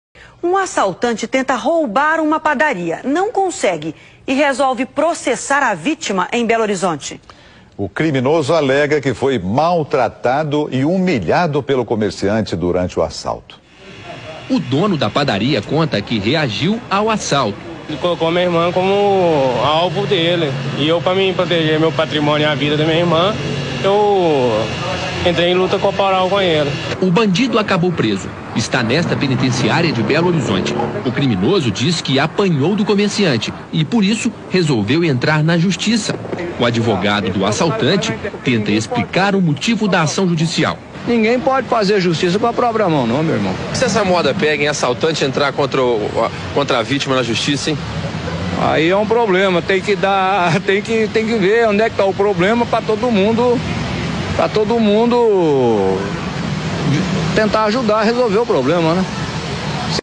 EXCLUDENTE DE ILICITUDE
ESTRANGEIRO, AQUI NO BRASIL LADRÃO ACIONA O PODER JUDICIÁRIO CONTRA SUA PRÓPRIA VÍTIMA DE ASSALTO

E Ainda Tem Advogado Que Incentiva A Aventura Judicial.
Cadê A Admoestação Verbal E Extinção dO Processo Sem Resolução dO Mérito, Sr. Juiz?
Sem Se Esquecer dAs Custas, Viu?
Hehehehehe